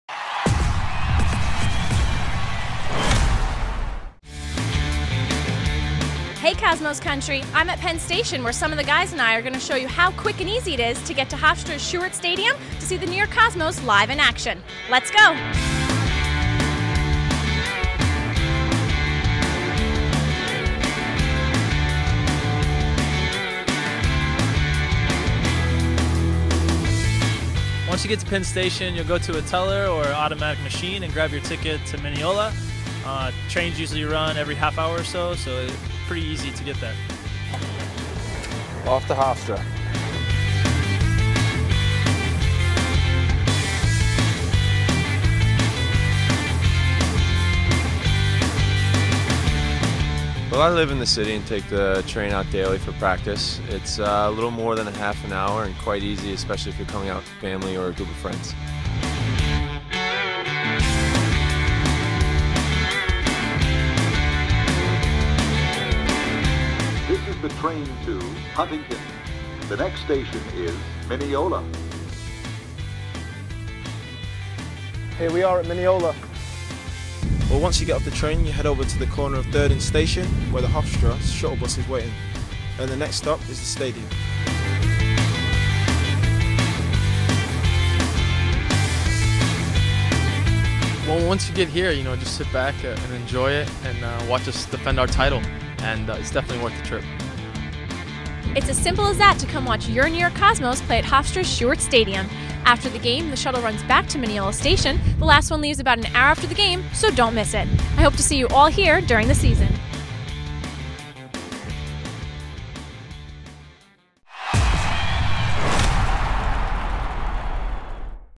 NY Cosmos: Getting to the Game via the LIRR
Getting to Hofstra University's Shuart Stadium from Manhattan is extremely easy. Check out the video to see just how fast you can be cheering on your New York Cosmos!